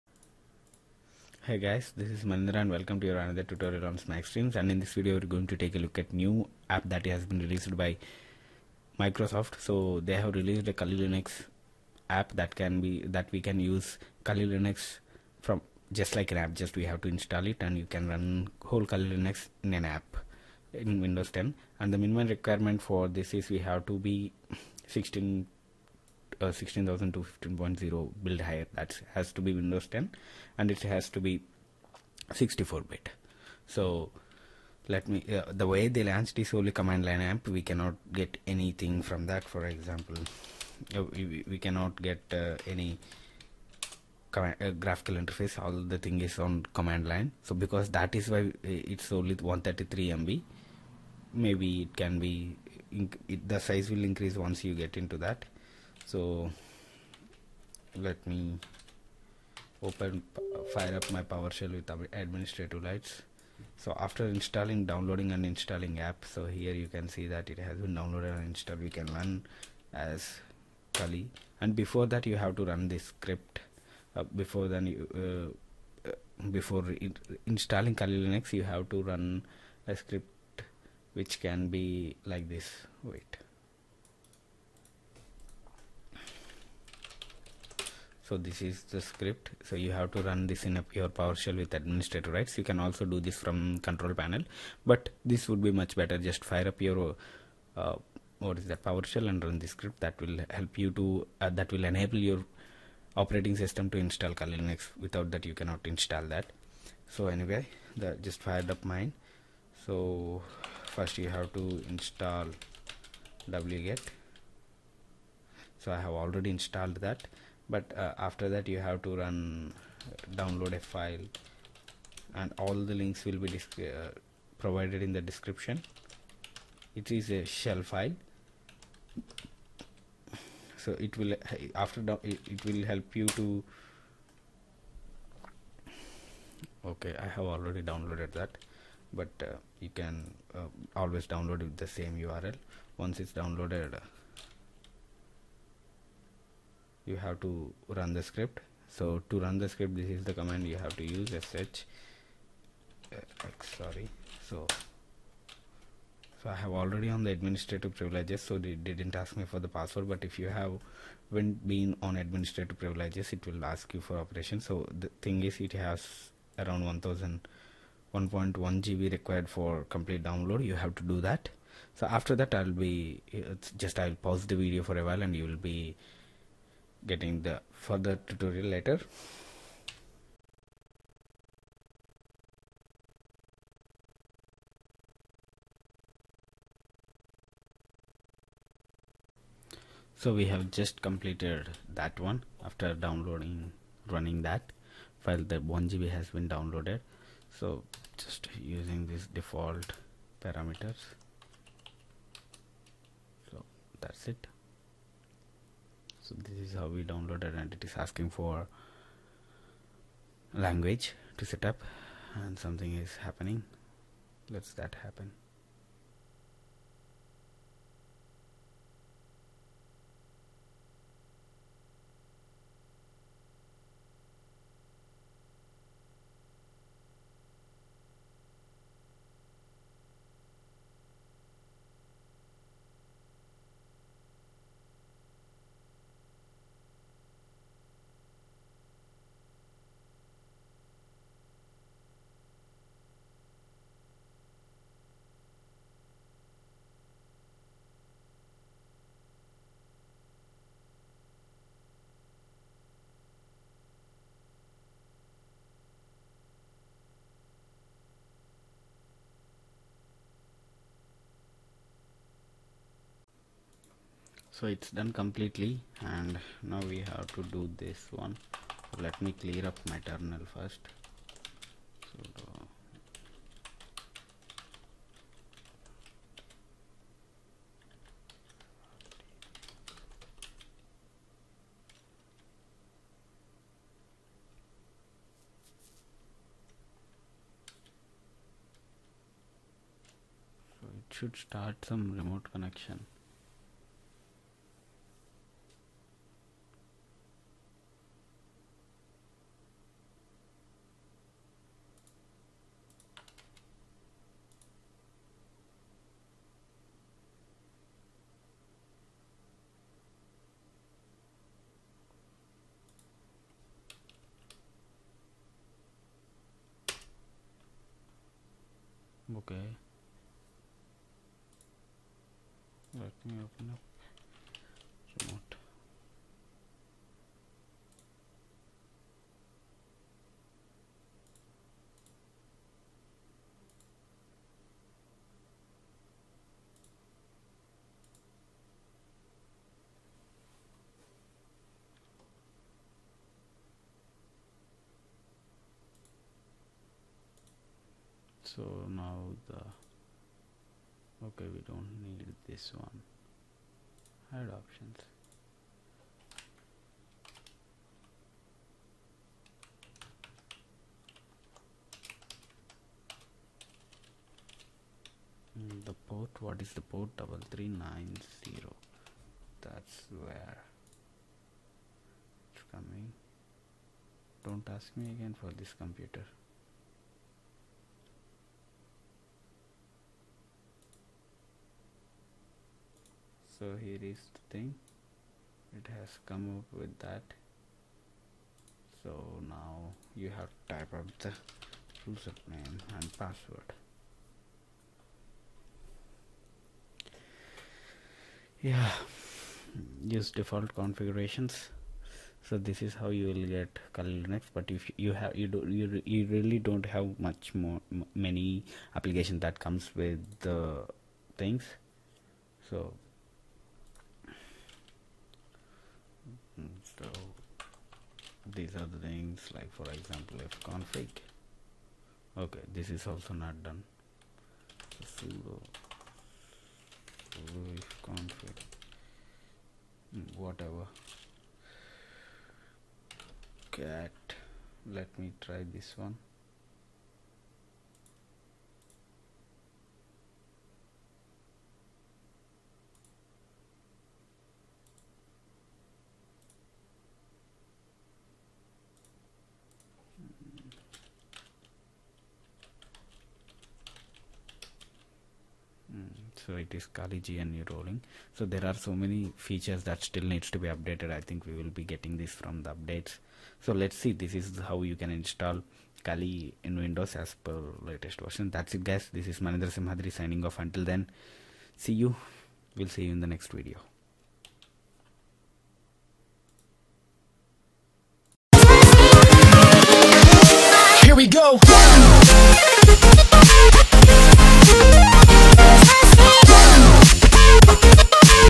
Install Kali Linux On Windows via Microsoft Store
Run this command in powershell(Admin):
Enable-WindowsOptionalFeature -Online -FeatureName Microsoft-Windows-Subsystem-Linux

For more details, cool tech and hacking tutorials